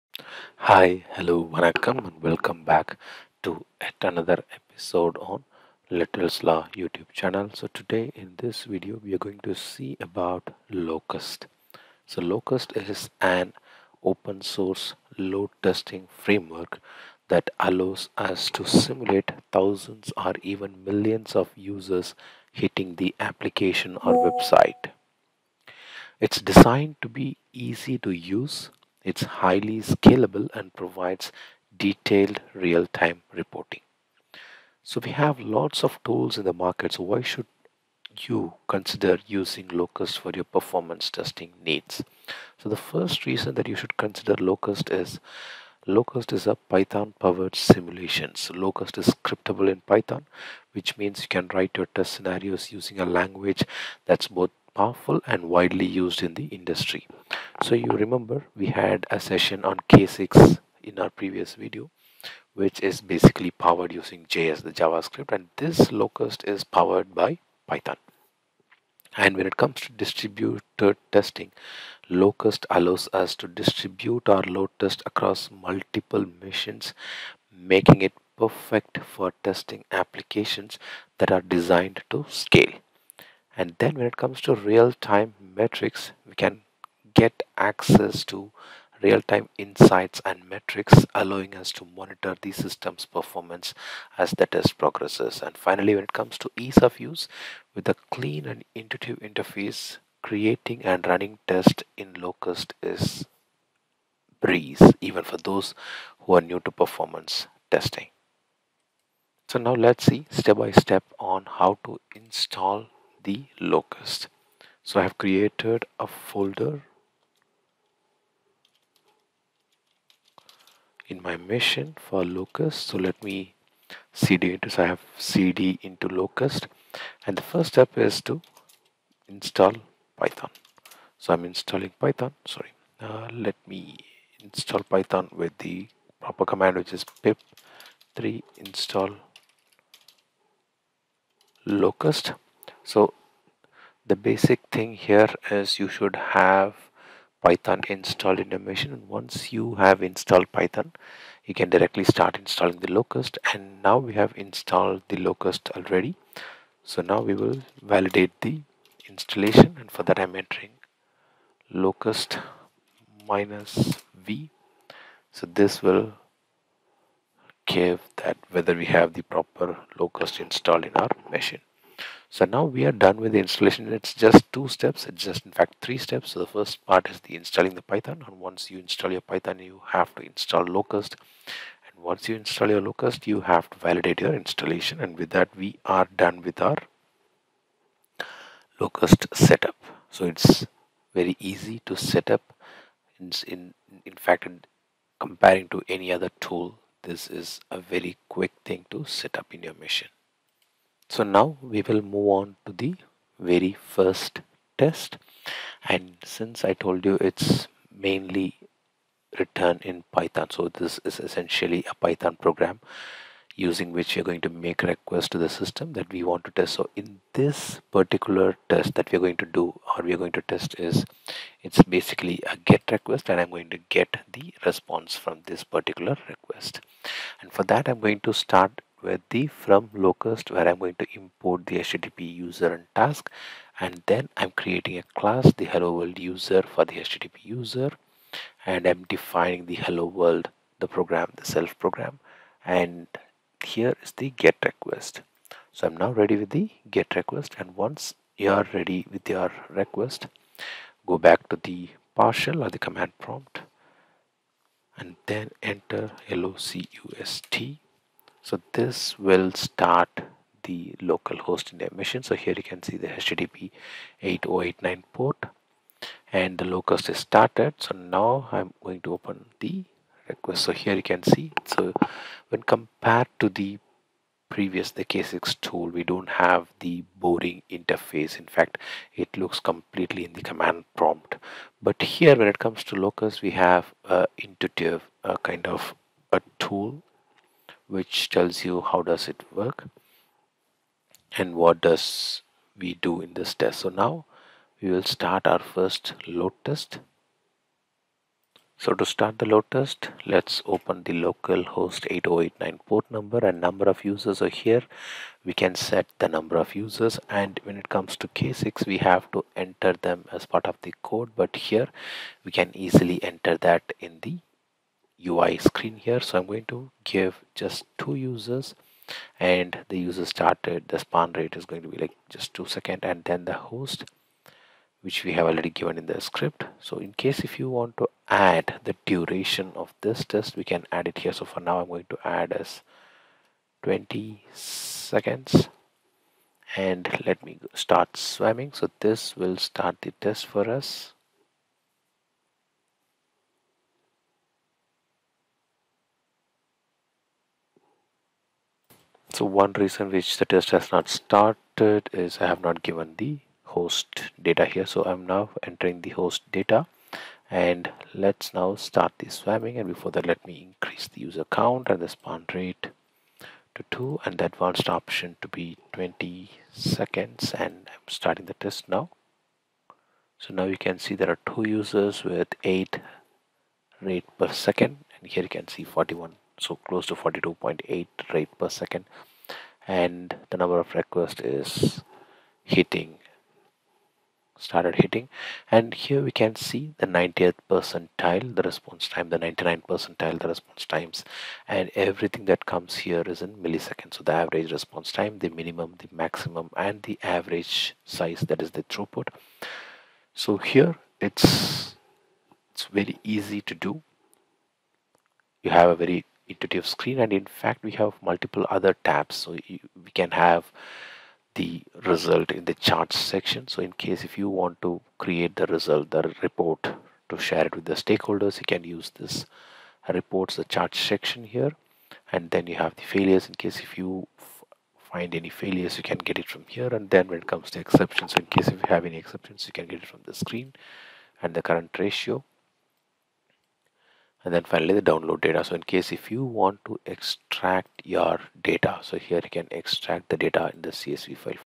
Introduction to Locust Performance testing Tool | Installation | First Test
Title: Getting Started with Locust: Installation and First Test

Description:

Welcome to Littles Law Youtube Channel ! In this video, we're diving into the powerful world of performance testing using the Locust tool. Whether you're a developer, tester, or simply someone interested in load testing, this tutorial will walk you through the installation process and help you set up your first test with Locust.

🔧 Installing Locust

0:00 - Introduction
0:42 - Step 1: Download Locust
1:12 - Step 2: Installing Locust (Windows)
2:03 - Verifying the Installation

🚀 Your First Test with Locust

2:30 - Setting Up a Simple Test Scenario
3:10 - Defining User Tasks (Python Script)
4:25 - Running Your Test
5:15 - Monitoring and Interpreting Results

By the end of this tutorial, you'll have Locust up and running on your system and will have successfully conducted your first performance test.

🌐 Helpful Links:

Locust Documentation
📌 Timestamps:
00:00 - Introduction
00:42 - Installing Locust
02:30 - Your First Test with Locust

📚 Resources:

Sample Test Scripts
Locust User Guide